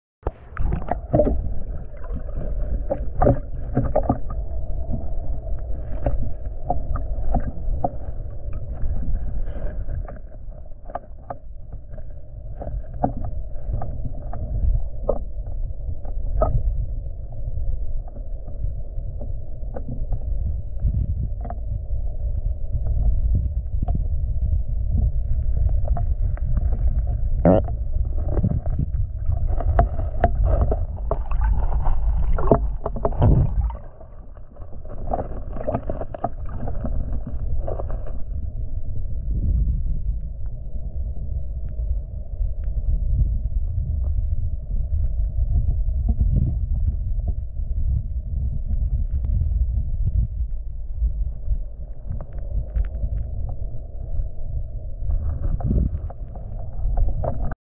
Diving In The Shannondale River Exploring.
Diving down 6 foot to see what's at the bottom of the Shannondale River. Surprised how clean it looked with scarious pollution.  Was hoping to find some old bottles or other objects but it looked pretty cleaned out.